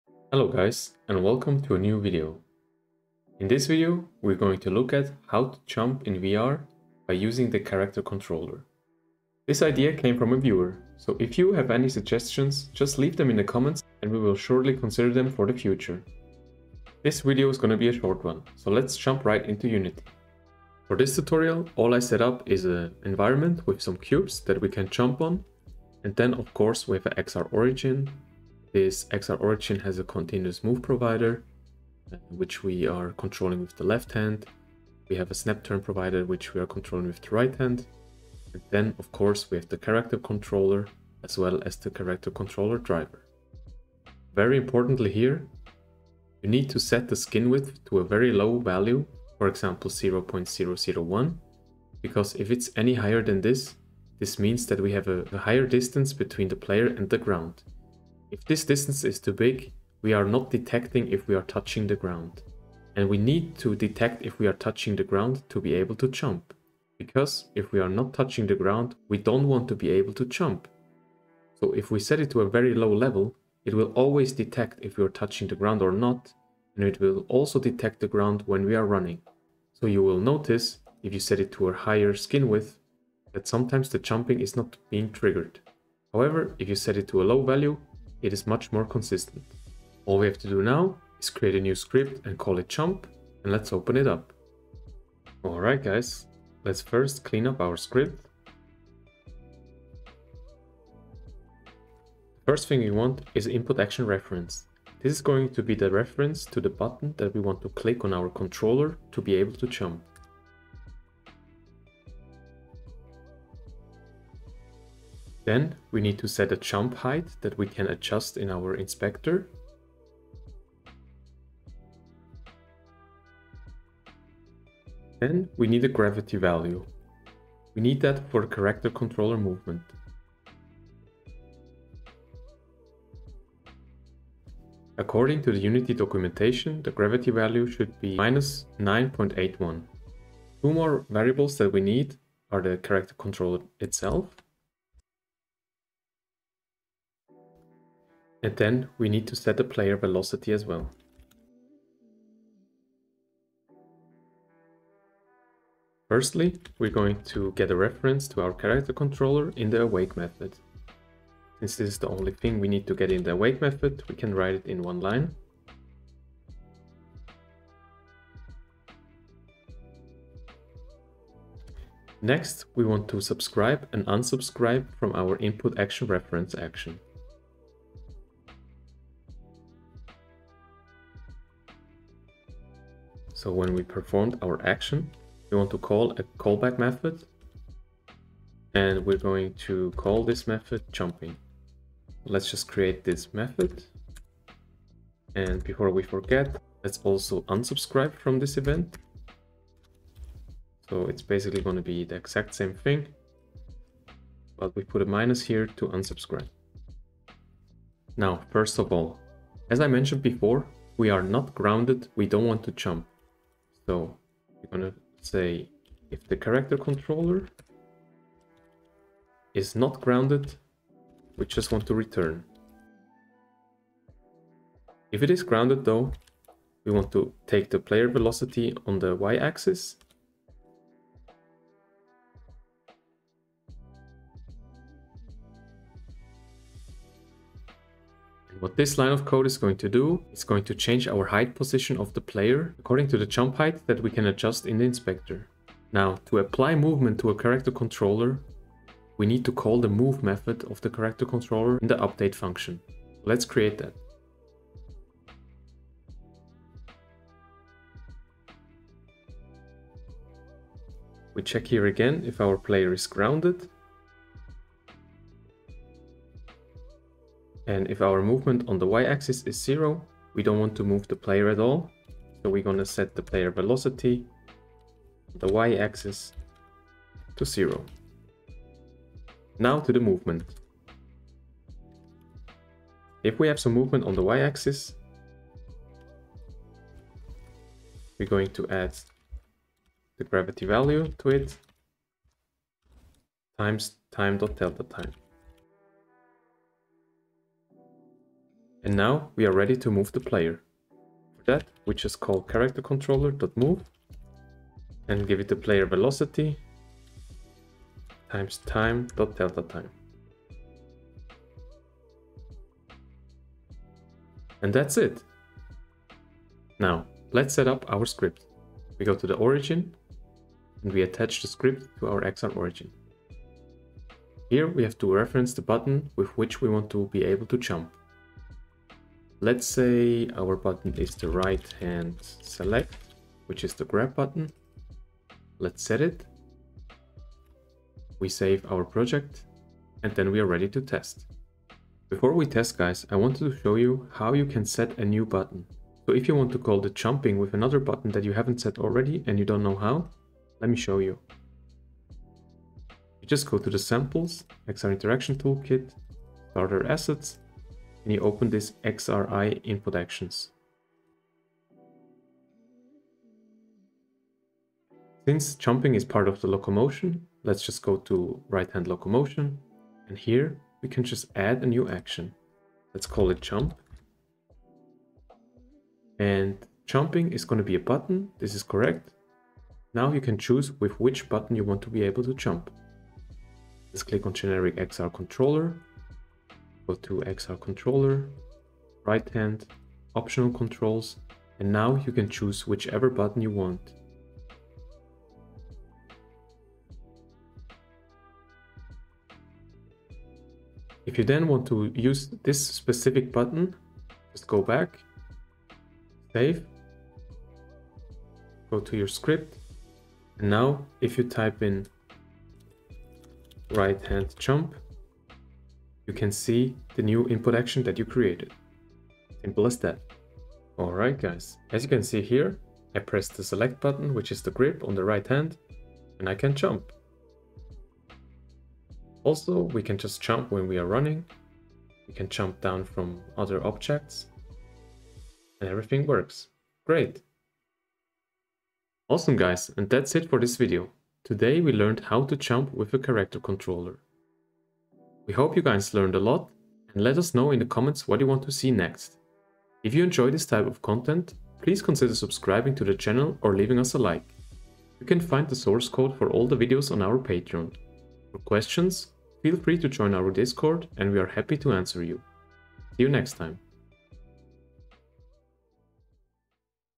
Jump in VR using the Unity Character Controller - Advanced VR Tutorials
In this video we are going to show you how to jump with your XR Origin using the Character Controller and CharacterController.Move.

We hope you are enjoying this video, if so, please consider supporting us below!

Resources:
🔗 CharacterController.Move:

Chapters:
0:00 Intro
0:20 Unity Setup
0:43 Character Controller Settings
1:33 Jump Script
5:16 Setup the Script in the Scene
5:42 Add additional InputActionReference (optional)
7:01 Testing
7:18 Outro